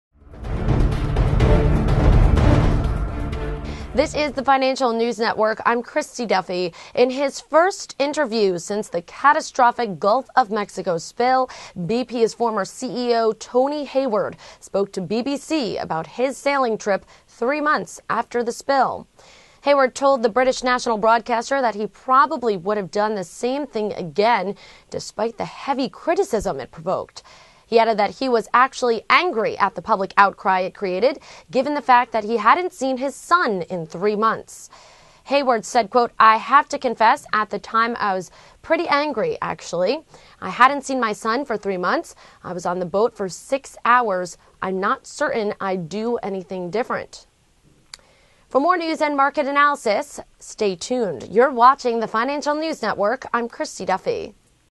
BP CEO Tony Hayward Finally Speaks Up About Heated Sailing Trip Subject
"In his first interview since the catastrophic Gulf of Mexico oil spill, BP's (NYSE:BP) former CEO Tony Hayward, spoke to BBC about his sailing trip three months after the spill.

Hayward told the British national broadcaster that he probably would have done the same thing again, despite the heavy criticism it provoked. He added that he was actually angry at the public outcry it created, given the fact that he hadn't seen his son in three months.

Hawyard said, ""I have to confess, at the time I was pretty angry actually. I hadn't seen my son for three months. I was on the boat for six hours ... I'm not certain I'd do anything different,"" "